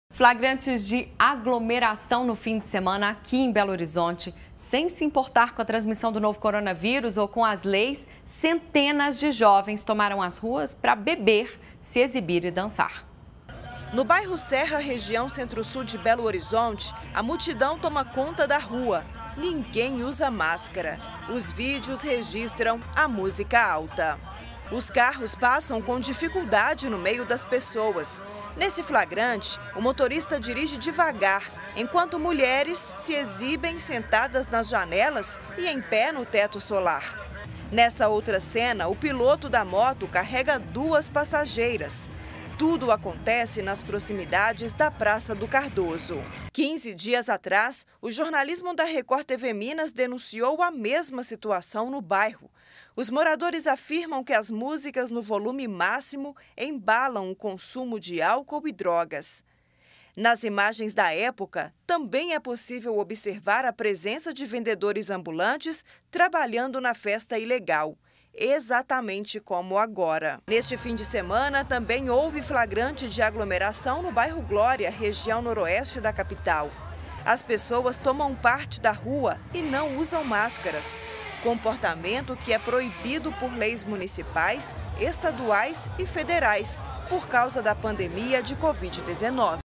Duas mulheres têm carros roubados por suspeitos em Venda Nova (MG)
Duas mulheres tiveram os carros levados por bandidos na região metropolitana de Belo Horizonte. Nos dois casos, os veículos ainda não foram recuperados.